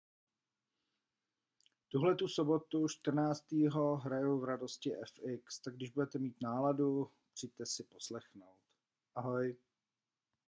DJ Loutka a jeho shadow pozvánka do Radosti FX 14.5.2016
DJ Loutka zve na sobotu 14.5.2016 do Radosti FX, dále zahraje Dan Cooley. 2 x Absolut Vodka a Red Bull za 145 korun :)